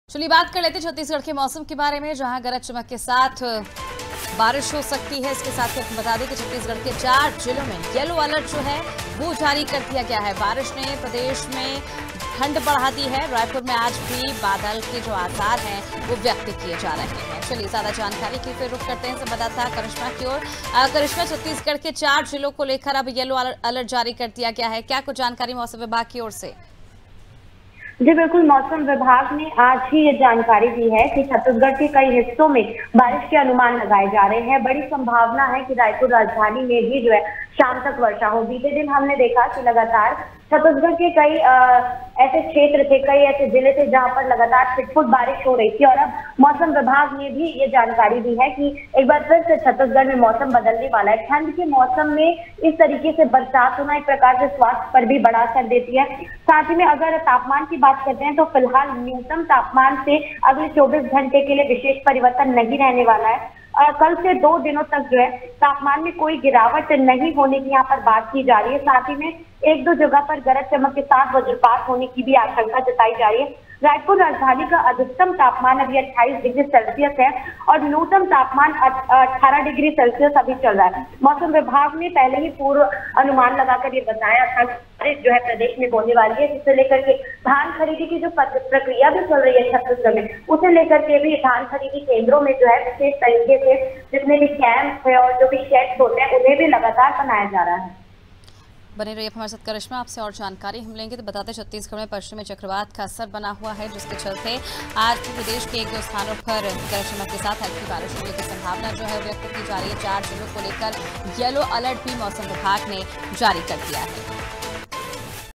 Raipur : छत्तीसगढ़ के 4 जिलों में यलो अलर्ट, गरज-चमक के साथ गिर सकते हैं ओले || Anaadi Tv
रायपुर:छत्तीसगढ़ के 4 जिलों में यलो अलर्ट
गरज-चमक के साथ गिर सकते हैं ओले
बारिश ने प्रदेश में बढ़ाई ठंड
रायपुर में आज भी बादल के आसार#breakingnews

AIRTEL - 379